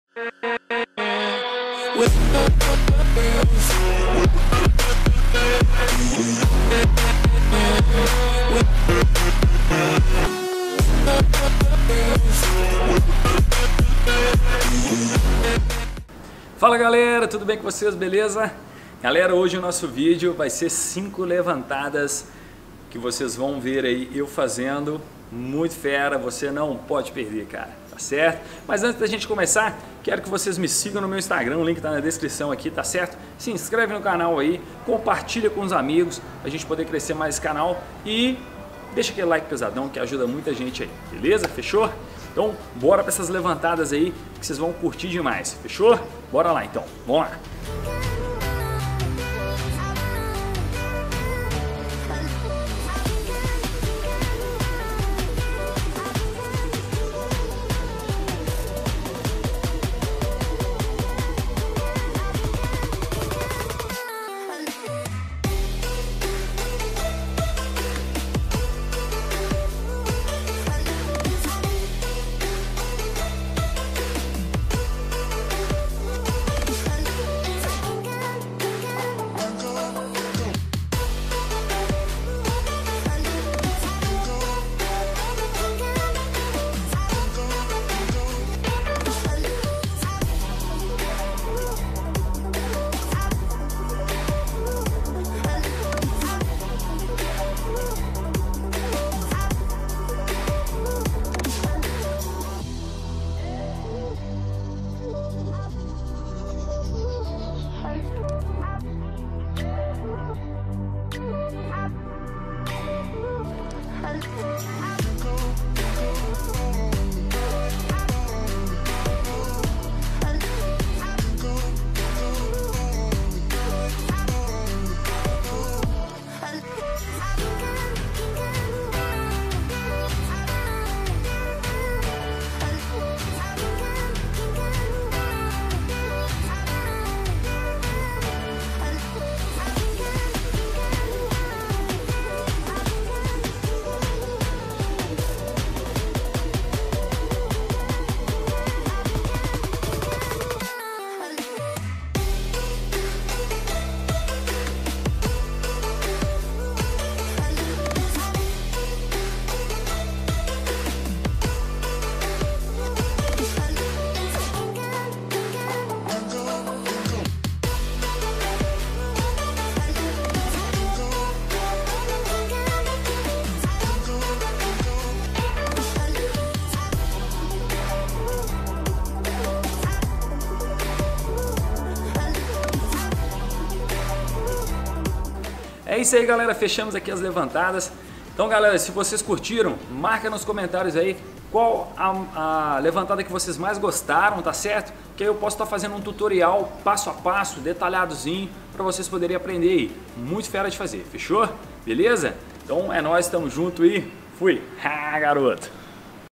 5 FORMAS DIFERENCIADAS PARA LEVANTAR A BOLA - FUTEBOL E FUTSAL
Fala galera, tudo bem com vocês? Vídeo de hoje são 5 FORMAS DIFERENCIADAS DE LEVANTAR A BOLA NO FUTEBOL E FUTSAL.